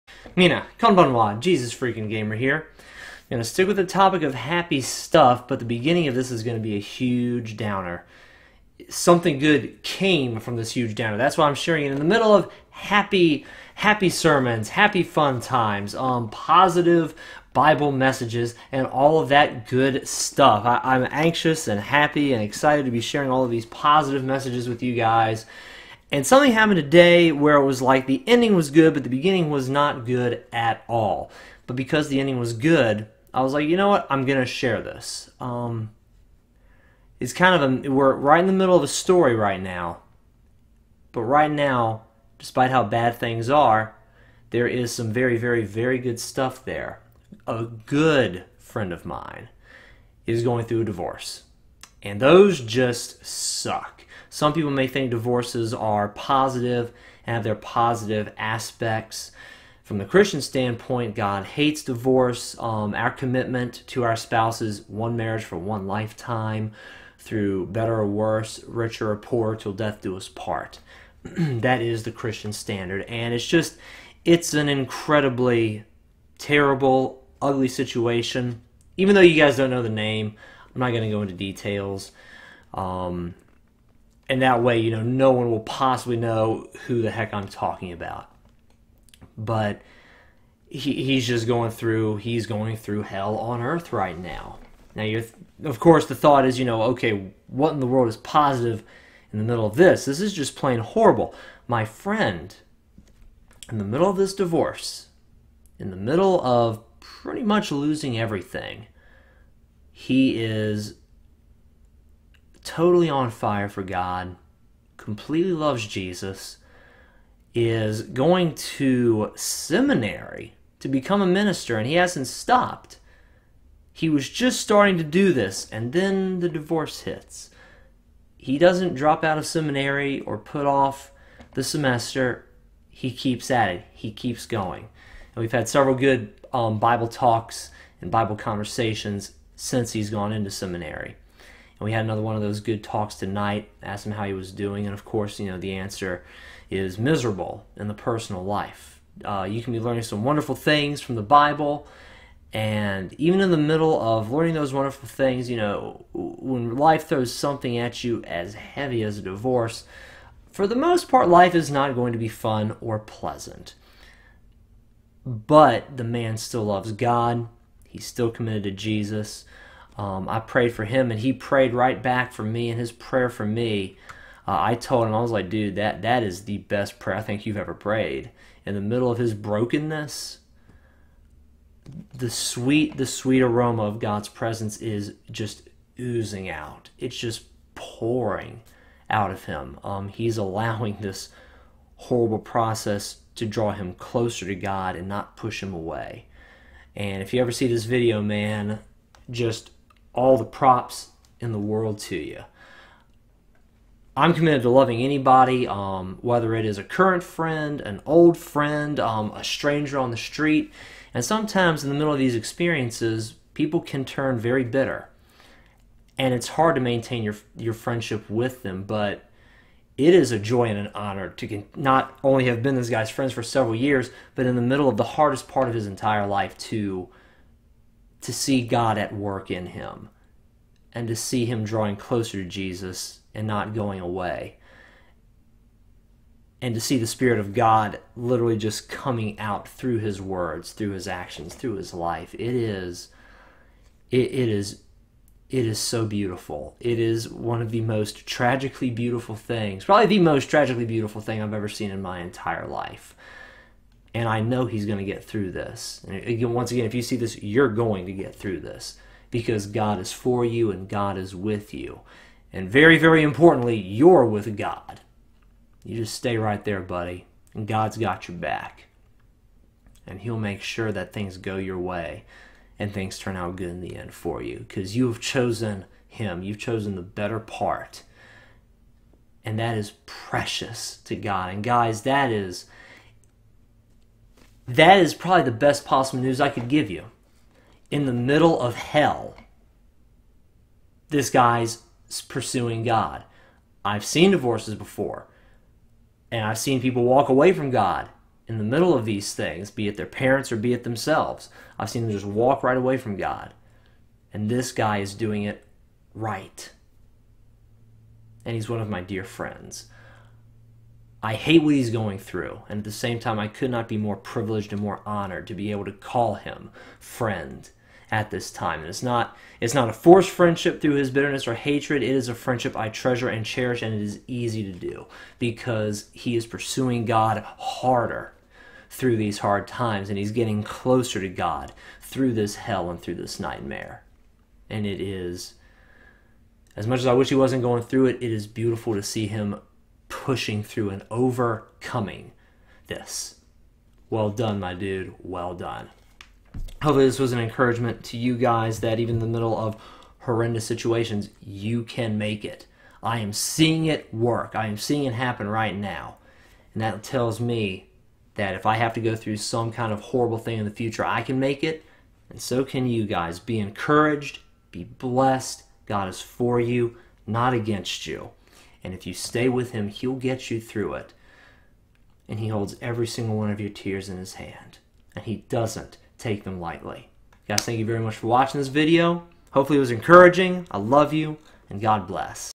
CONQUERING THE GIANT OF DIVORCE CAN BE DONE
It's not impossible. I'm seeing it be done in front of my eyes right now, so I know there's hope for anyone in Christ.